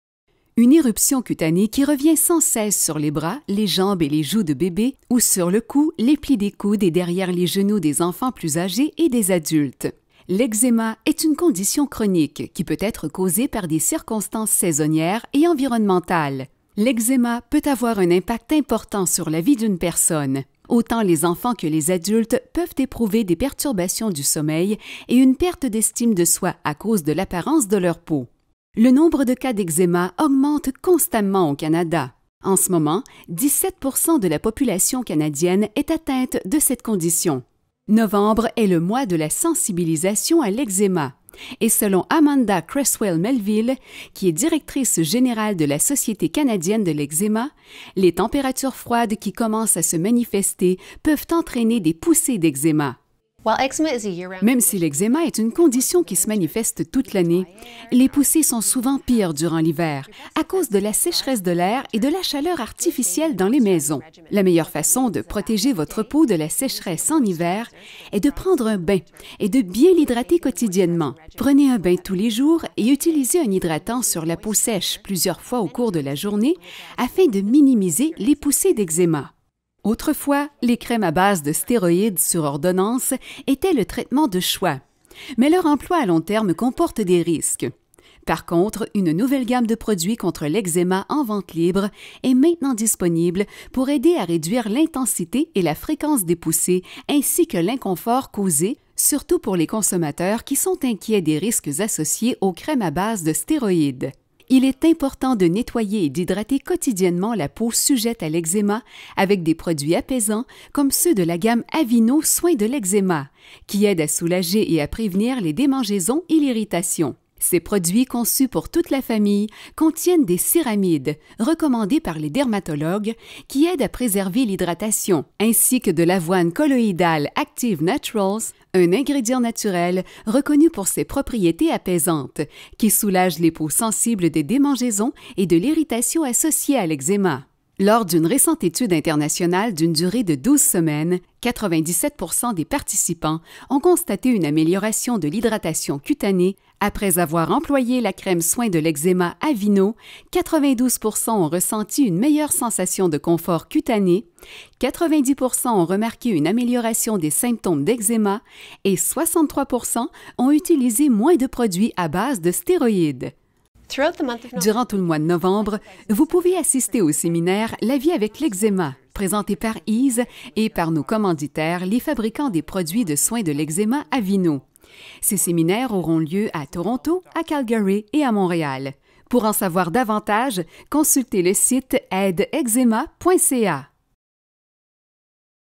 Aide pour l'eczéma
Nouvelle crème en vente libre pour traiter l'eczéma et hydrater la peau sèche qui démange. Contient des ingrédients naturels pour apaiser et soulager les irritations et les éruptions cutanées.